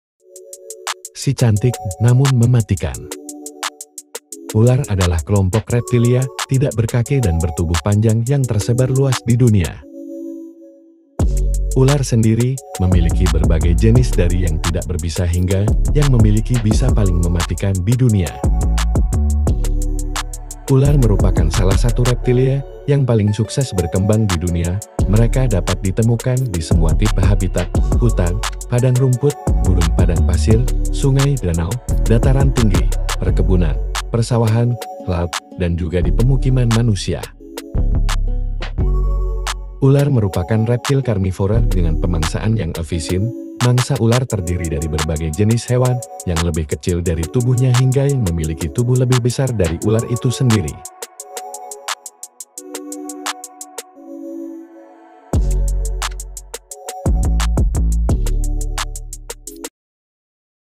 Sicantik Namun Mematikan
Ular adalah kelompok reptilia tidak berkaki dan bertubuh panjang yang tersebar luas di dunia.

Ular merupakan salah satu reptilia yang paling sukses berkembang di dunia. Mereka dapat ditemukan di semua tipe habitat: hutan, padang rumput, gurun/padang pasir, sungai, danau, dataran tinggi, perkebunan, persawahan, laut, dan juga di pemukiman manusia.

Ular merupakan reptil karnivora dengan pemangsaan yang efisien. Mangsa ular terdiri dari berbagai jenis hewan yang lebih kecil dari tubuhnya ataupun yang memiliki tubuh lebih besar dari ular itu sendiri